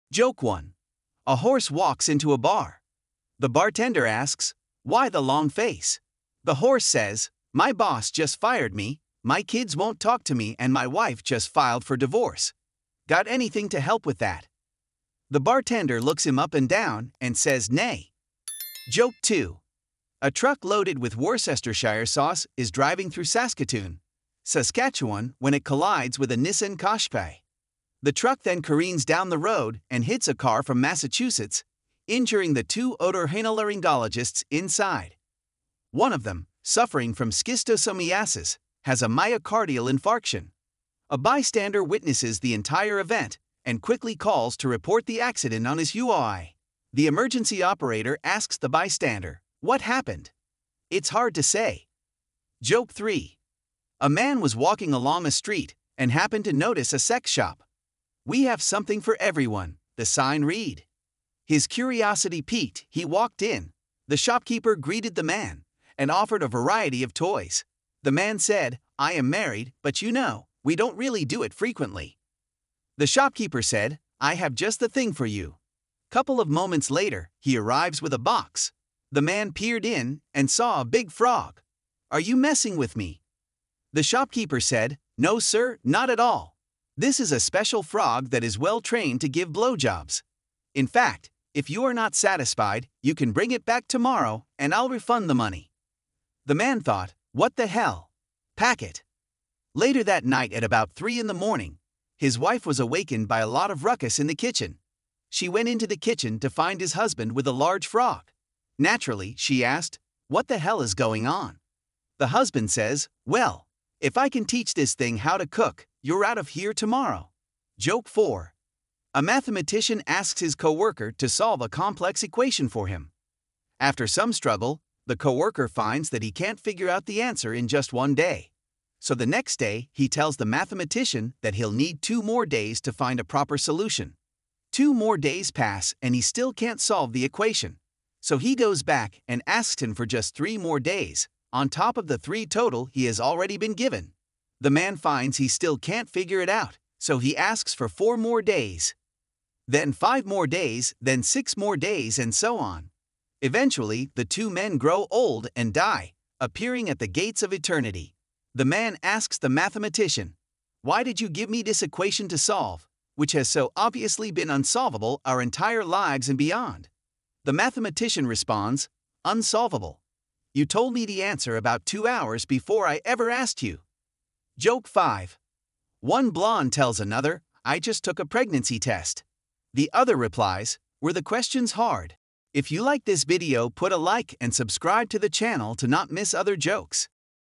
🤣 A man was walking along a street, and happened to notice a sex shop... | Funny Daily Jokes
🤣 BEST DAD'S JOKE! - A man was walking along a street, and happened to notice a sex shop... | Funny Daily Jokes

👇 THE JOKE 👇

Joke 1.
A horse walks into a bar..

The bartender asks, "Why the long face?"

The horse says, "My boss just fired me, my kids won't talk to me and my wife just filed for divorce. Got anything to help with that?"

The bartender looks him up and down, and says "Neigh."

Joke 2.
A truck loaded with Worcestershire sauce is driving through Saskatoon, Saskatchewan when it collides with a Nissan Qashqai.
The truck then careens down the road and hits a car from Massachusetts, injuring the two otorhinolaryngologists inside. One of them, suffering from Schistosomiasis, has a myocardial infarction.

A bystander witnesses the entire event and quickly calls to report the accident on his Huawei.

The emergency operator asks the bystander, "What happened?"

"It's hard to say."

Joke 3.
A man was walking along a street, and happened to notice a sex shop.

'We have something for everyone!', the sign read.

His curiosity piqued, he walked in. The shopkeeper greeted the man, and offered a variety of toys. The man said, 'I am married, but you know, we don't really do it frequently'.

The shopkeeper said, 'I have just the thing for you.'

Couple of moments later, he arrives with a box. The man peered in, and saw a big frog. 'Are you messing with me?'

The shopkeeper said, 'No sir, not at all! This is a special frog that is well trained to give blowjobs. In fact, if you are not satisfied, you can bring it back tomorrow, and I'll refund the money.'

The man thought, 'What the hell. Pack it.'

Later that night at about 3 in the morning, his wife was awakened by a lot of ruckus in the kitchen. She went into the kitchen to find his husband with a large frog. Naturally, she asked, 'What the hell is going on?

The husband says, 'Well. If I can teach this thing how to cook, you're out of here tomorrow.'

Joke 4.
A mathematician asks his coworker to solve a complex equation for him.

After some struggle, the coworker finds that he can't figure out the answer in just one day, so the next day he tells the mathematician that he'll need two more days to find a proper solution.

Two more days pass and he still can't solve the equation, so he goes back and asks him for just three more days, on top of the three total he has already been given. The man finds he still can't figure it out, so he asks for four more days.

Then five more days, then six more days, and so on.

Eventually the two men grow old and die, appearing at the gates of Eternity. The man asks the mathematician: "Why did you give me this equation to solve, which has so obviously been unsolvable our entire lives and beyond?"

The mathematician responds: "Unsolvable? You told me the answer about 2 hours before I ever asked you!"

Joke 5.
One blonde tells another "I just took a pregnancy test"

The other replies: "Were the questions hard?"